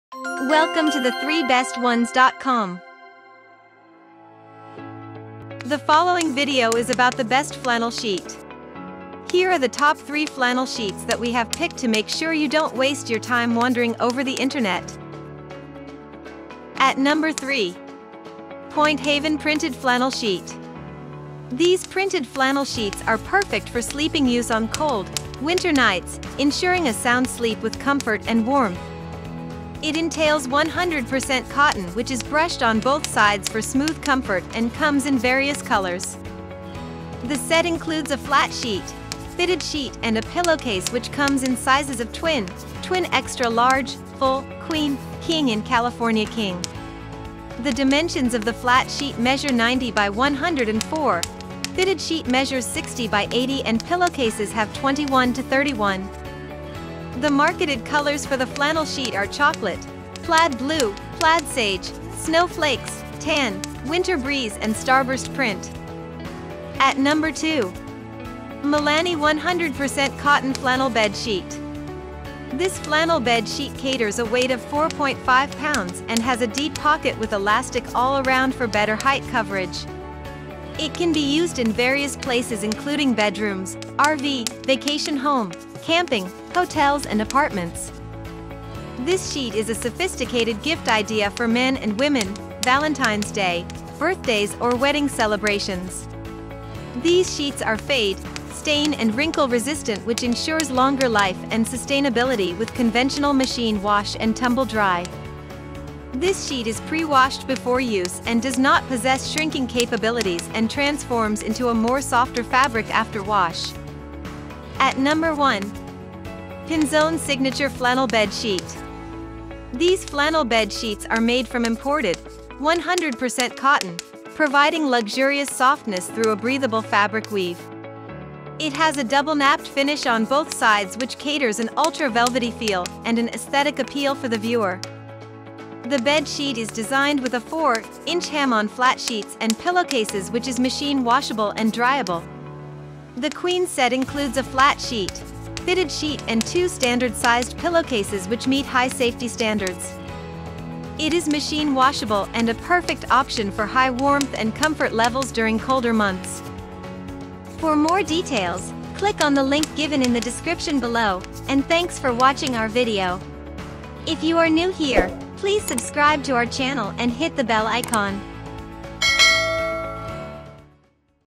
Best Flannel Sheets In 2023
Follow the links given below to check the product prices:

Here is the list of the top 3 best "Flannel Sheets":

Pinzon Signature Flannel Bed Sheet:

Mellanni 100% Cotton Flannel Bed Sheet:

Pointehaven Printed Flannel Sheet:

0:00 The Best Flannel Sheets According To Reviews
0:16 Pointehaven Printed Flannel Sheet
1:06 Mellanni 100% Cotton Flannel Bed Sheet
01:54 Pinzon Signature Flannel Bed Sheet
02:37 Closing Remarks & Subscribe To Our Channel